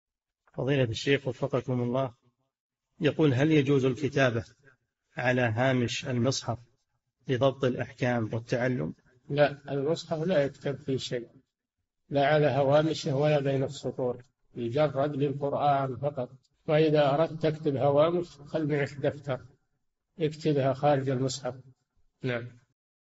[4534- 4717] هل يجوز الكتابة على هامش المصحف لضبط الأحكام؟ - الشيخ صالح الفوزان
"[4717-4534] هل يجوز الكتابة على هامش المصحف لضبط الأحكام والتعلم؟
الجَوَابُ: لا، المصحف لا يكتب فيه شيء، لا على هوامشه ولا بين السطور، يجرد للقرآن فقط، وإذا أردت أن تكتب هوامش فخذ معك دفترًا واكتبها خارج المصحف.

للاشتراك في قناة الشيخ صالح الفوزان
فتاوى المجالس في تفسير المفصل

للاستفسار والإقتراحات والدعم
رئيس الفريق
لمشروع كبار العلماء